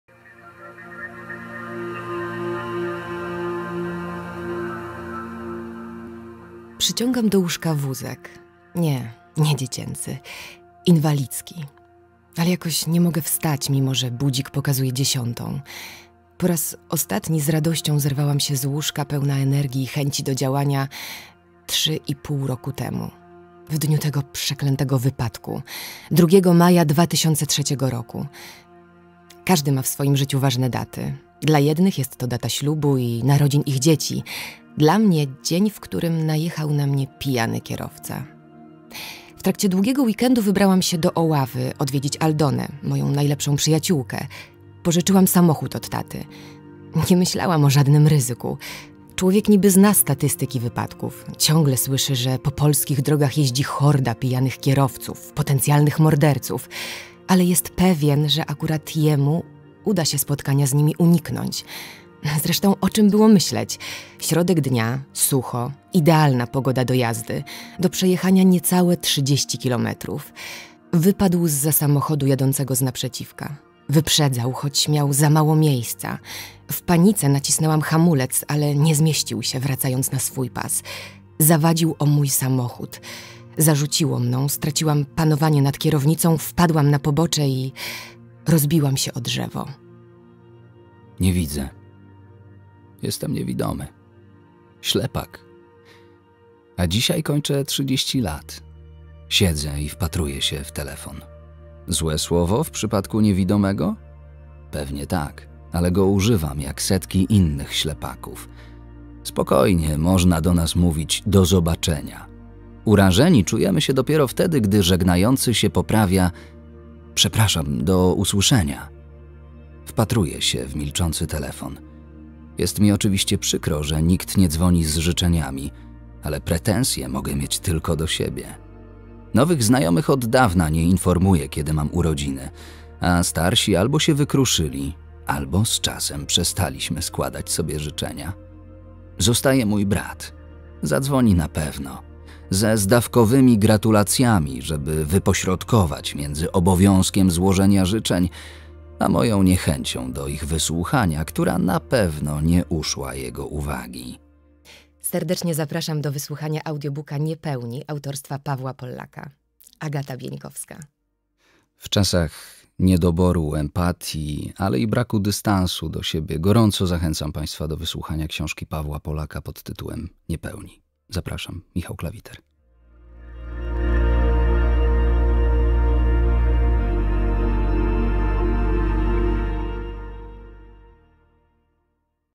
PAWEŁ POLLAK "Niepełni" (audiobook promo)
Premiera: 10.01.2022
Czyta: Agata Bieńkowska, Michał Klawiter
Opis:
To wzruszająca historia o poszukiwaniu miłości. Edyta po wypadku porusza się na wózku inwalidzkim. Marzy, by wyjść za mąż i mieć dziecko. Jacek, niewidomy od urodzenia, świetnie zarabia jako informatyk i od kilku lat bezskutecznie szuka dziewczyny. Pewnego dnia ich losy się przecinają... Edyta i Jacek mają swoje marzenia, ambicje, plany, zainteresowania i chcą żyć pełnią życia a przede wszystkim chcą kochać i być kochani. Gdyby byli sprawni, ich pragnienia byłyby normalne, a tak otaczający świat pokazuje im, że oczekują od życia zbyt wiele...

@Lind&Co